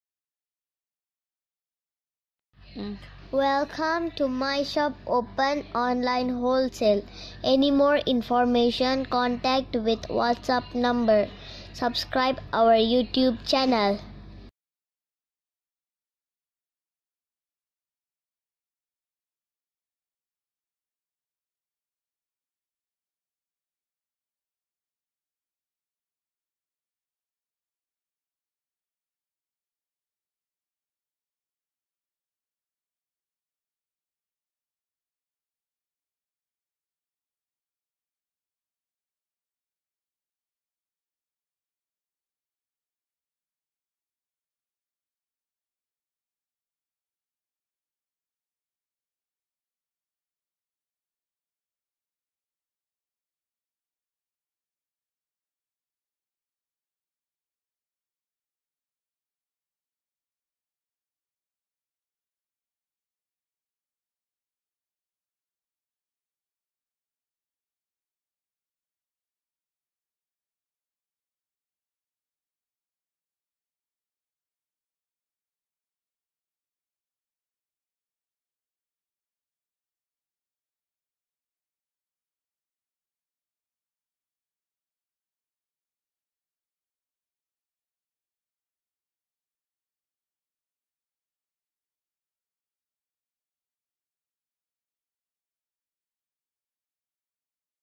MSO_SAREE_9530 : New Launch Heavy Rangoli Silk Beautiful Ruffle Designer Saree Collections
👉MSO_SAREE_9530 : New Launch Heavy Rangoli Silk Beautiful Ruffle Frill & Patch Butta N Saree with Beautiful Pearl & Flower Work Unstitched Blouse